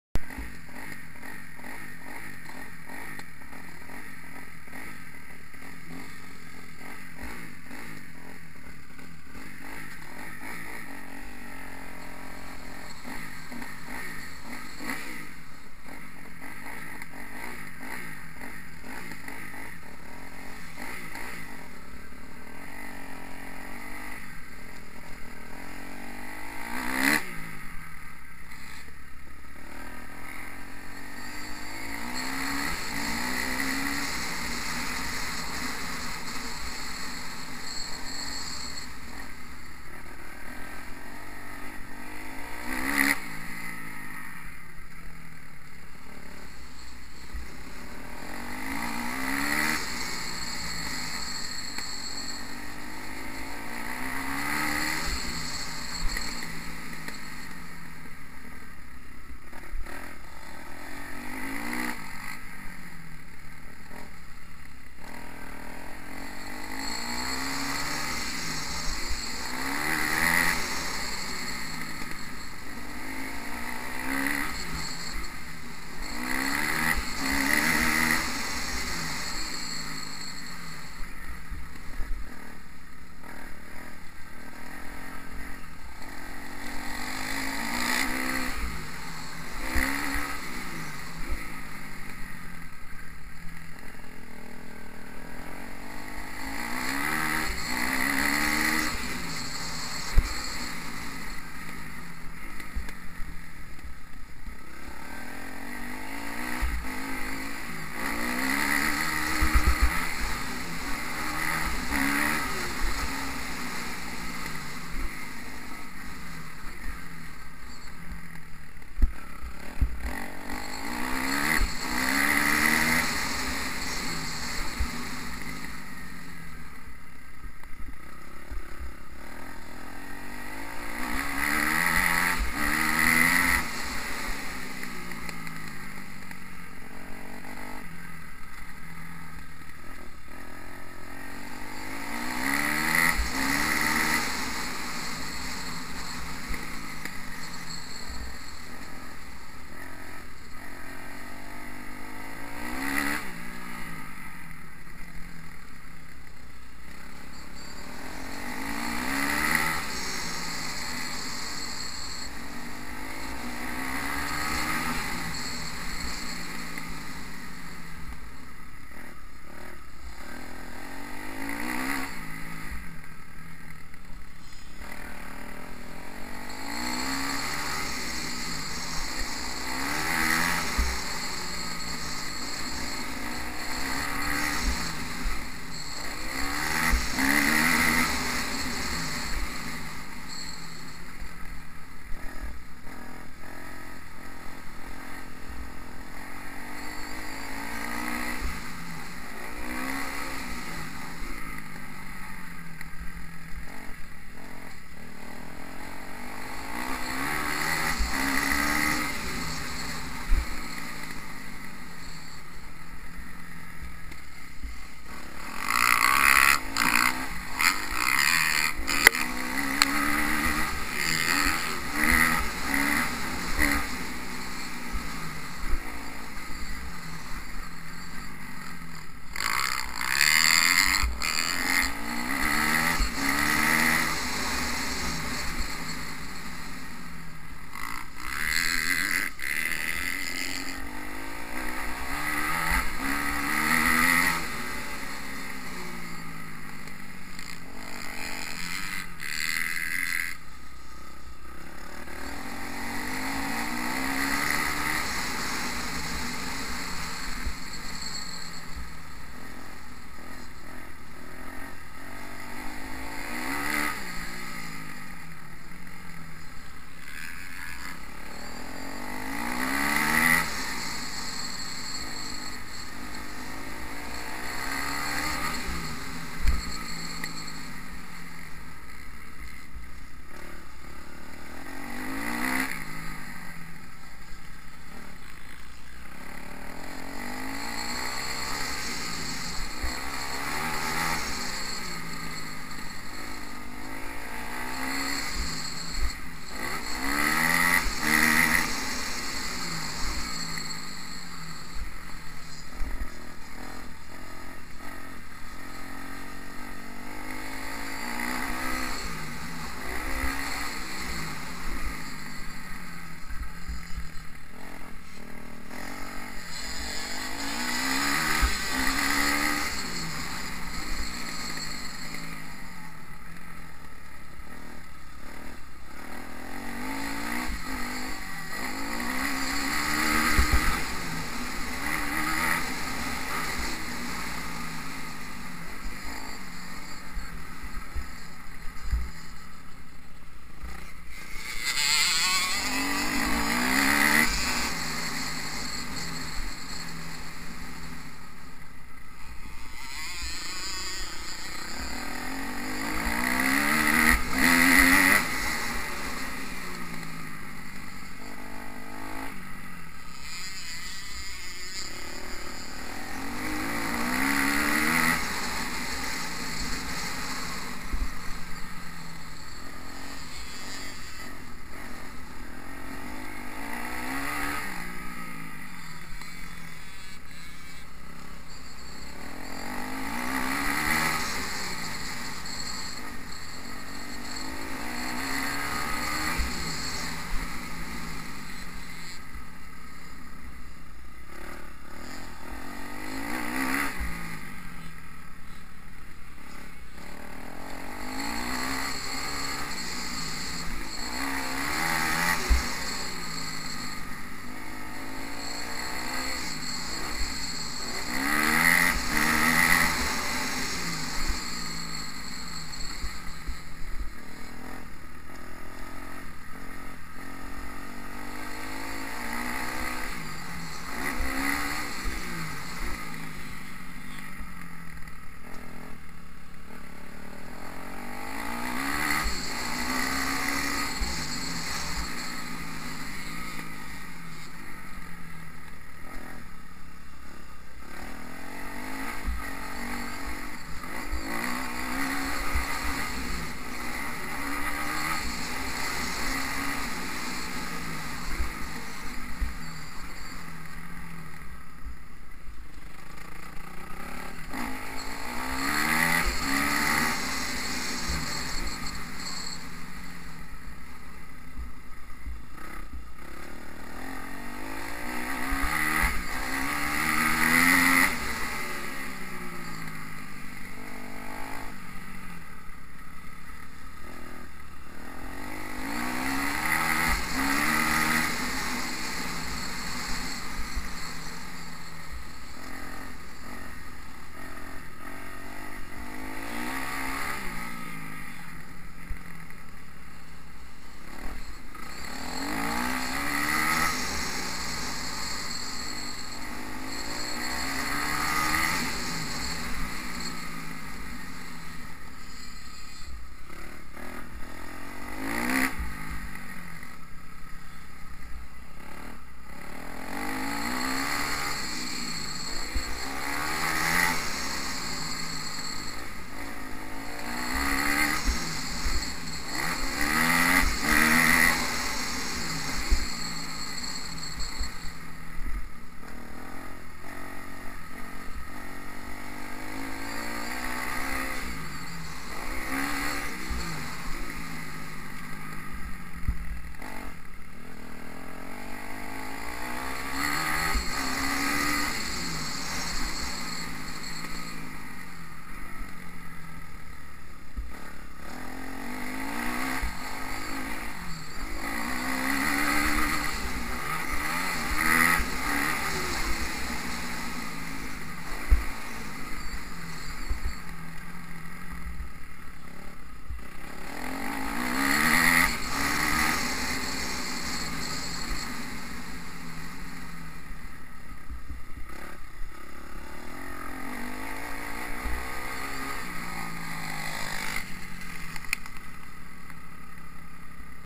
East trax 28/02/13
another day at east trax, getting more confident now but still a way to go.